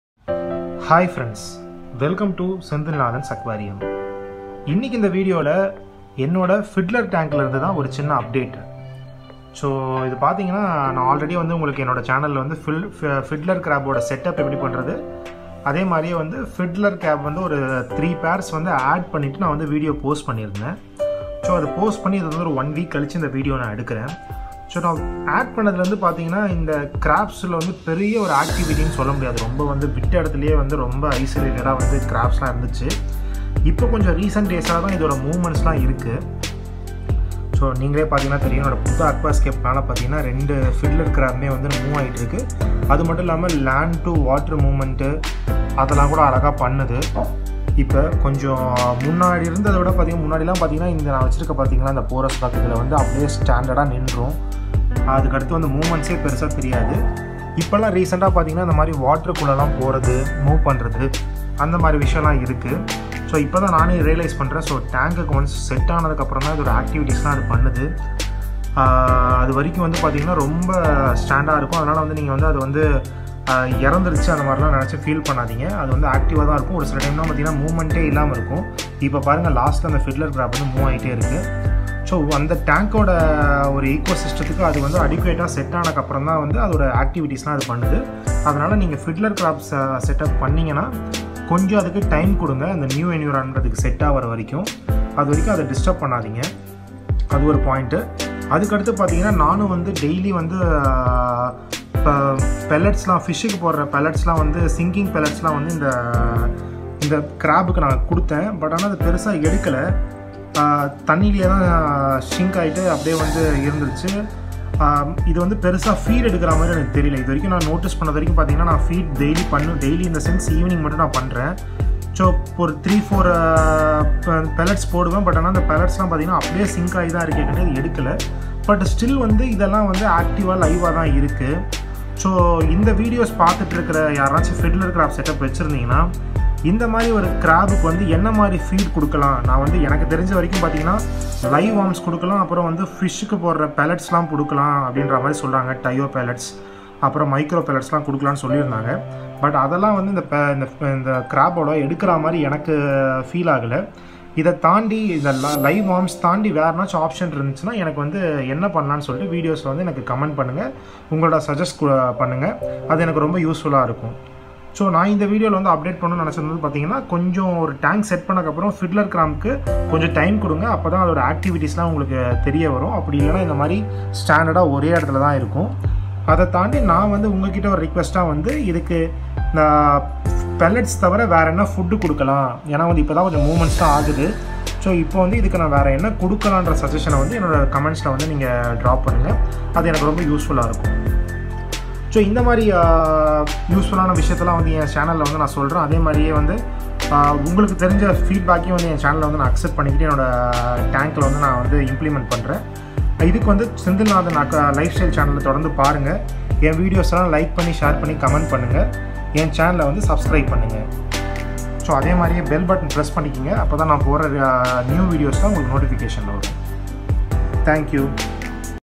Fiddler Crab Activity | Ecosystem | Suggestion needed for his feeding | Tamil
This video I have showed the fiddler Crab Activity after its get settled with the ecosystem. Also I have requested the food suggestion for fiddler Crab since my Crab is not taking any of the fish pellets.

Do watch the video fully and suggest me some good foods which Crab can consume. Don’t forget to share the feedback in comment section.